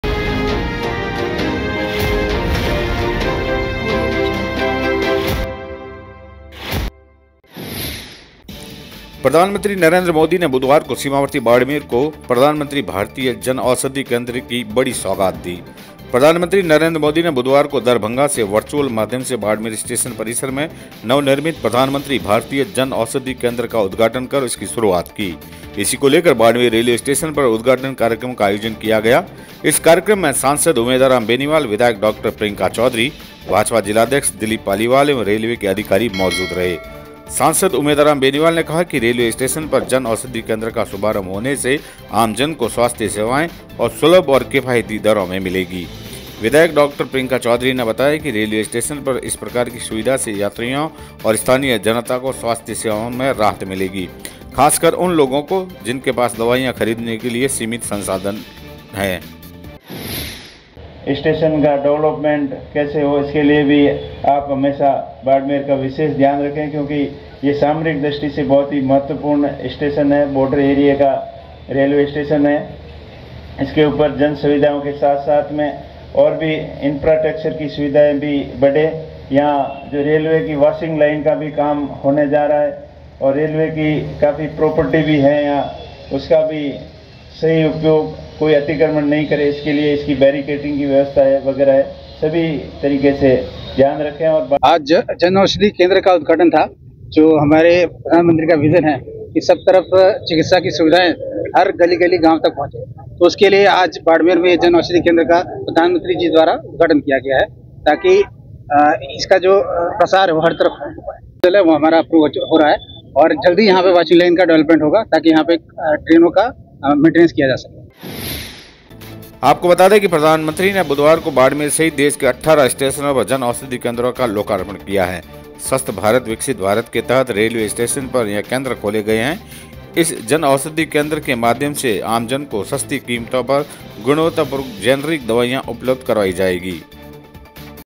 बाड़मेर रेल्वे स्टेशन पर प्रधानमंत्री भारतीय जनऔषधि केंद्र का उद्घाटन, कईं अतिथियों ने की शिरकत
बाड़मेर रेल्वे स्टेशन पर प्रधानमंत्री भारतीय जनऔषधि केंद्र का हुआ उद्घाटन, कार्यक्रम में कईं अतिथियों ने की शिरकत